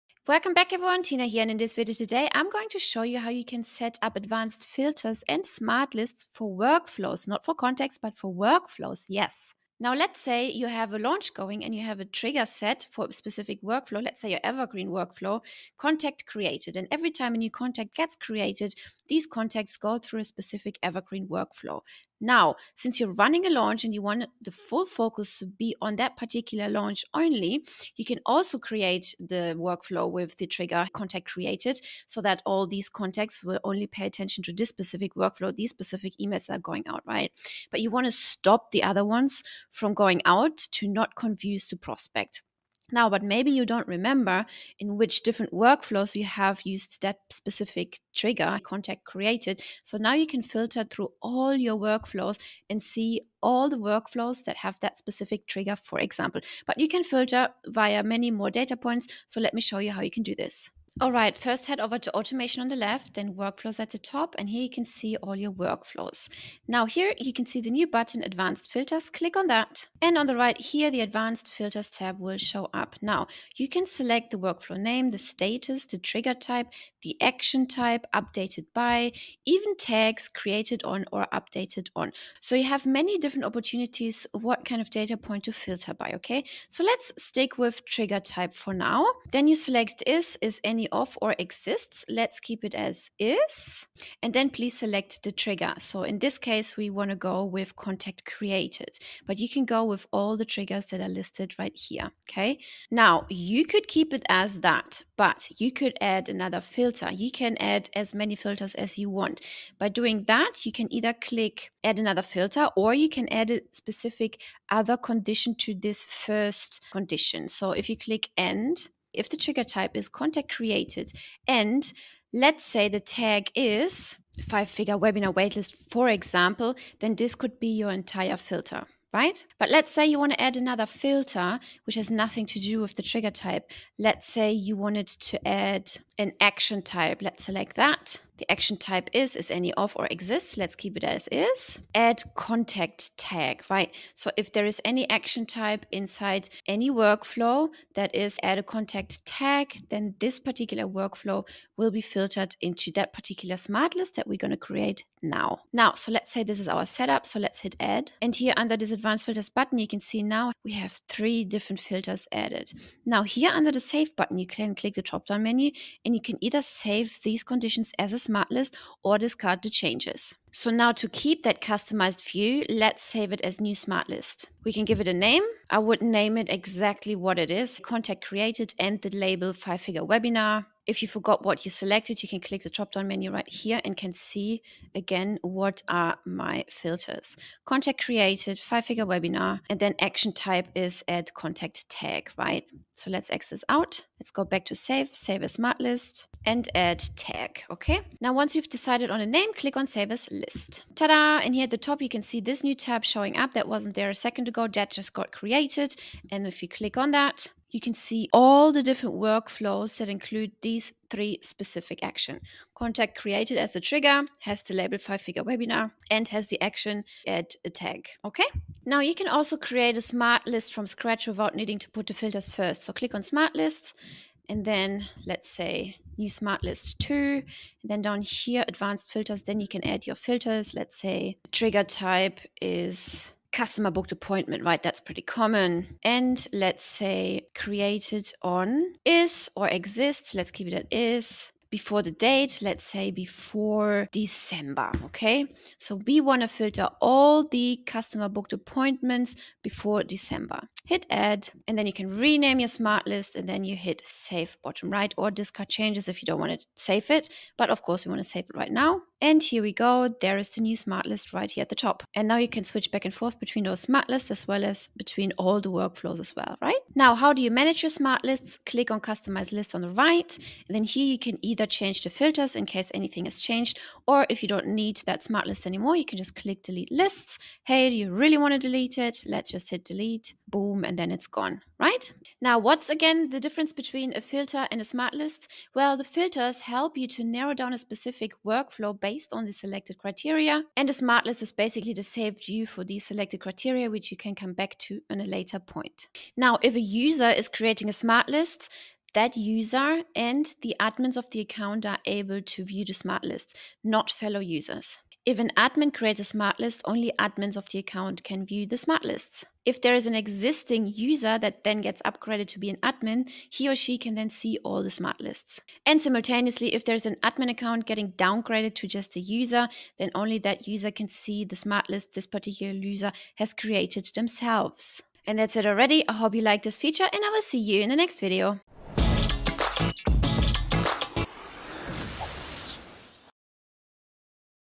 Workflow Automation: Setting Up Advanced Filters and Smart Lists
In this video, Tina explains how to set up advanced filters and smart lists for workflows. She walks through the steps to create and customize filters, save them as smart lists, and manage these lists effectively. Learn how to navigate through different criteria and understand the differences between filters and smart lists.

00:00 Introduction to Advanced Filters and Smart Lists
00:09 Setting Up Workflow Triggers
01:00 Using Advanced Filters
02:42 Creating and Saving Smart Lists
04:26 Managing and Deleting Smart Lists
04:42 Understanding Filters vs. Smart Lists
04:56 Admin and User Permissions for Smart Lists
05:25 Conclusion and Final Thoughts